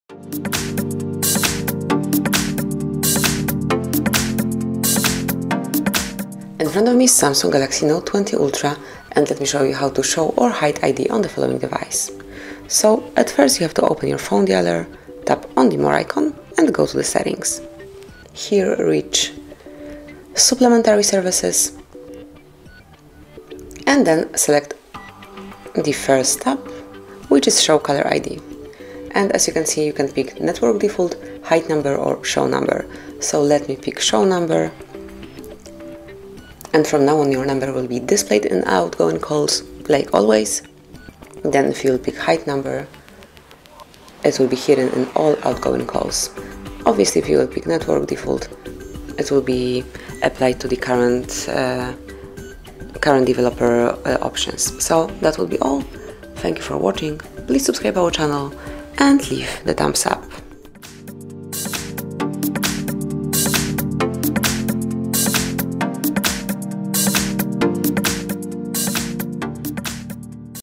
How to Show & Hide Caller ID in SAMSUNG Galaxy Note 20 Ultra – Call Settings
Read more info about SAMSUNG Galaxy Note 20 Ultra:
We are coming to present the video instructions, where you will be able to learn how to show or even hide caller ID in SAMSUNG Galaxy Note 20 Ultra. If you are looking for a way to make your number private, you definitely should watch the uploaded video guide and get access to the call settings, where you will be able to successfully hide and show your phone number. Let’s use our tutorial and enter calling settings to customize them easily. Visit our HardReset.info YT channel and discover many useful tutorials for SAMSUNG Galaxy Note 20 Ultra.